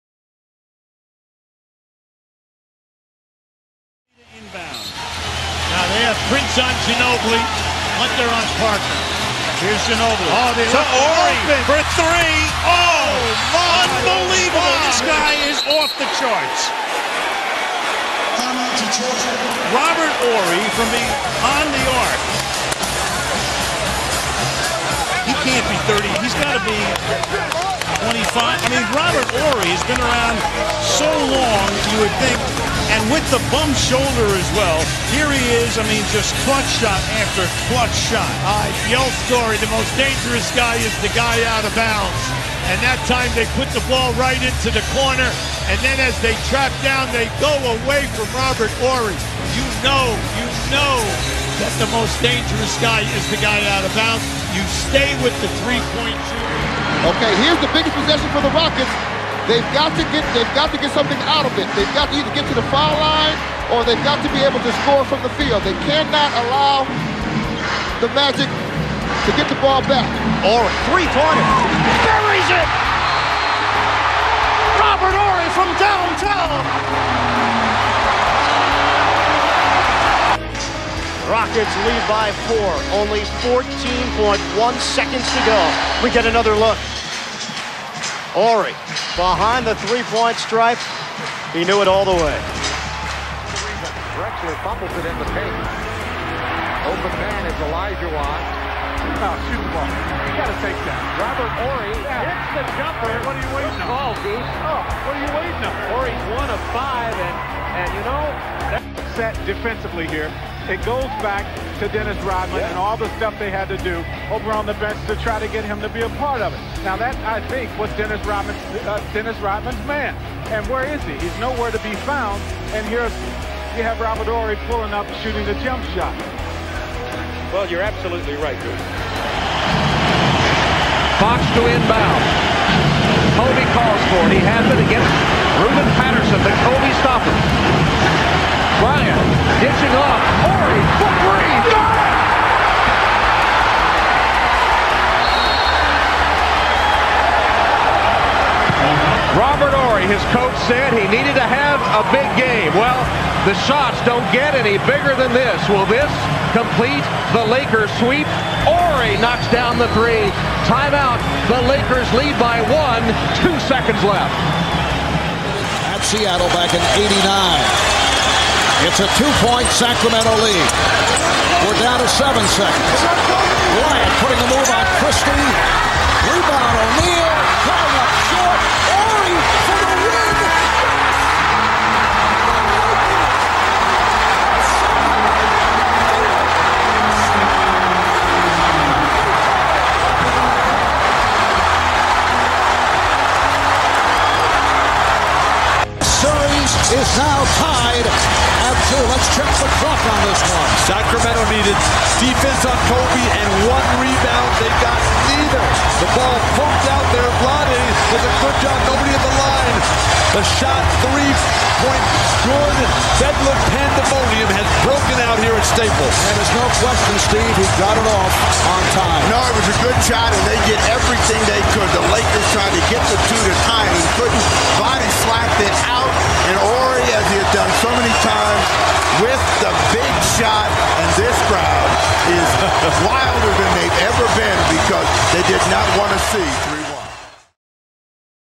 ROBERT HORRY TOP 5 CLUTCH SHOTS/BUZZER BEATERS OF CAREER!
ROBERT HORRY top 5 clutch SHOTS/BUZZER BEATER of career.

Thank you all for the support, good vision.